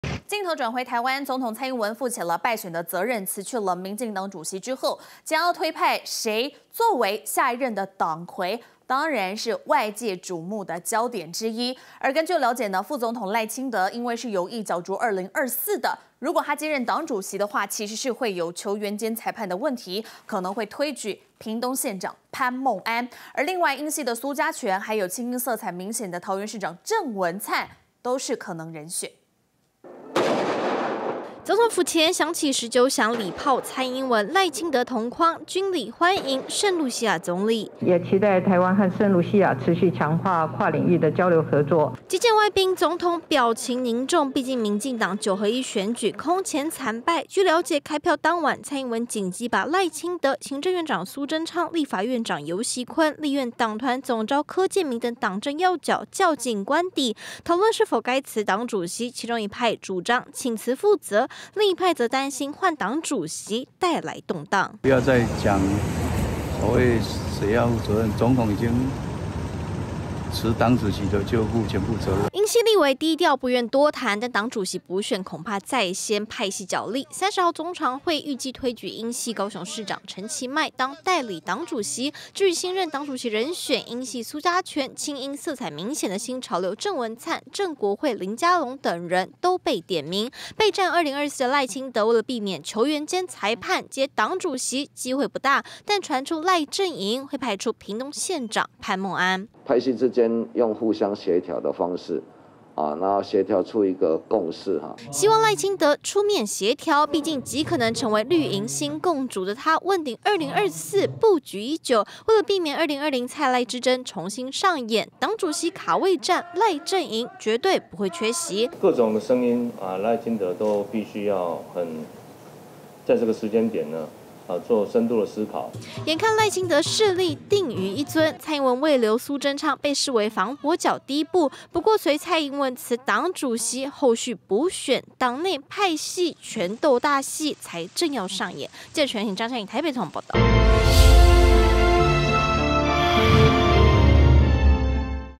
民進黨慘敗誰接掌？ 傳英蘇系不放權 賴系且戰且走
總統蔡英文負起敗選責任，辭去民進黨主席後，將推派誰作為下一任黨魁，仍有許多猜測。據了解，副總統賴清德因為有意角逐2024，接黨主席有球員兼裁判的問題，可能推舉屏東縣長潘孟安來角逐。另外英系蘇嘉全、親英色彩明顯的桃園市長鄭文燦都是可能人選。